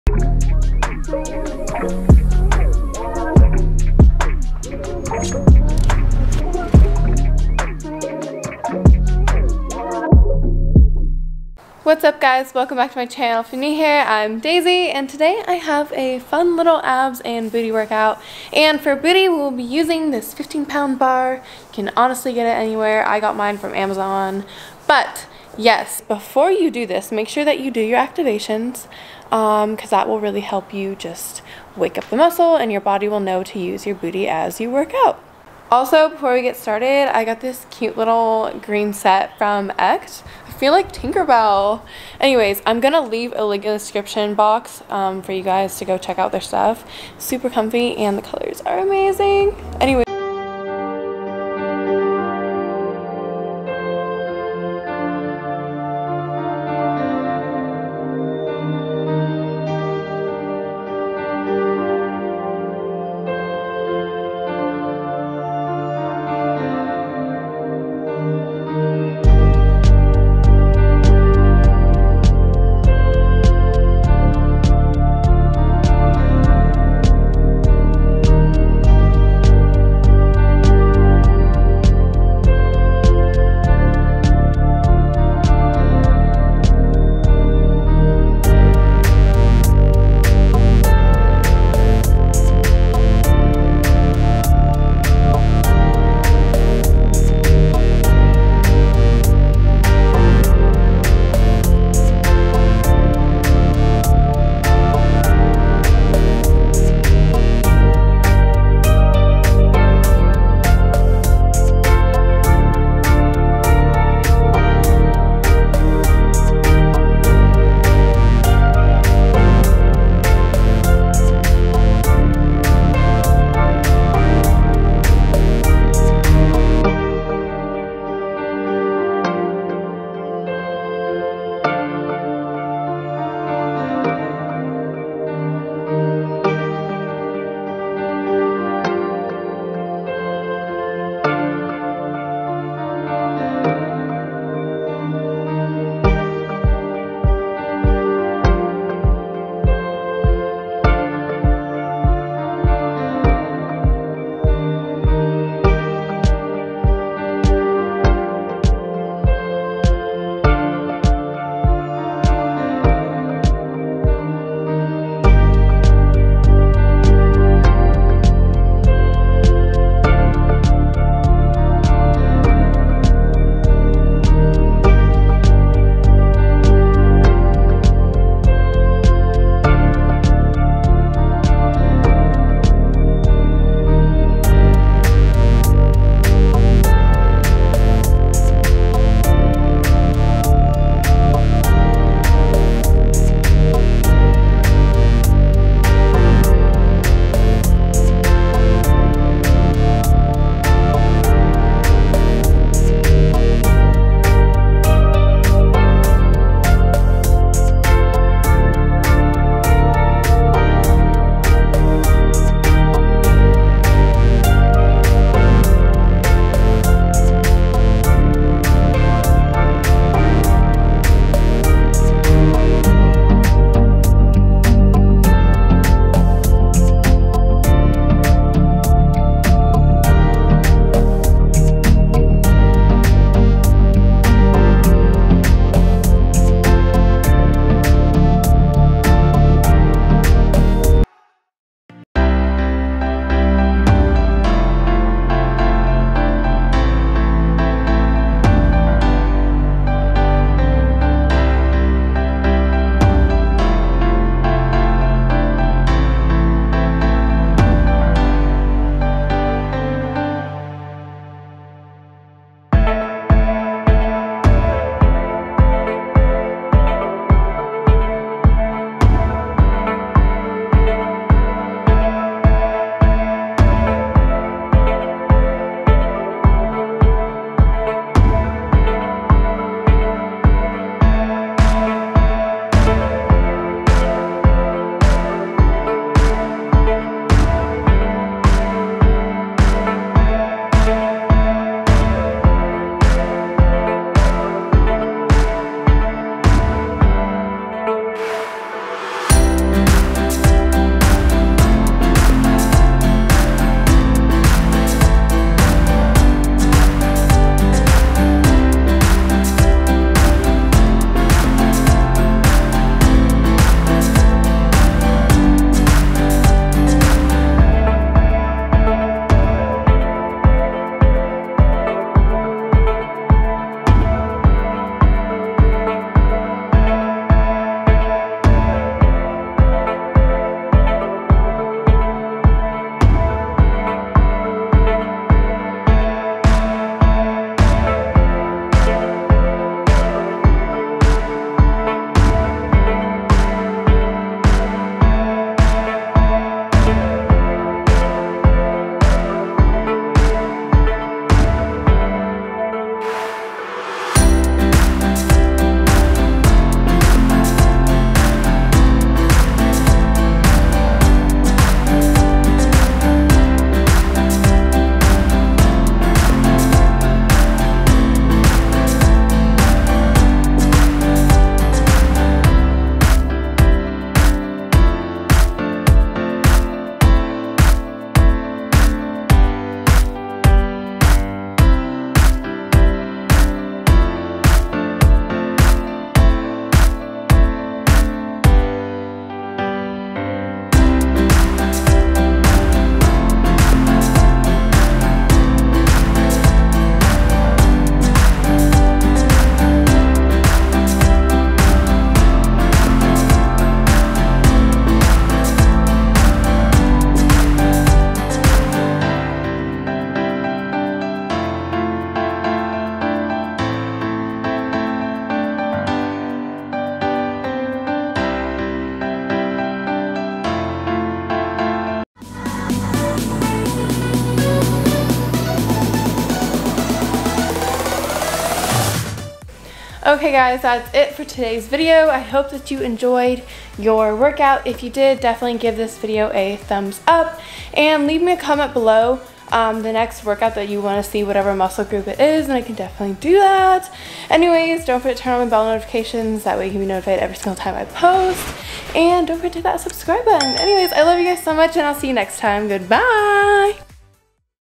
At Home Booty & Abs Circuit | 7 Minutes
Shop Cute Workout Sets 💚

Hi Peaches!

Here is another one of my favorite at home circuits for booty & abs!  Don't forget to do your activation exercises before the circuit! Repeat the circuit 3-5 times for more difficulty! Drop a ☀️  in the comments if you are setting goals and working on your summer body still!

Comment a workout you would like to see in the comments below!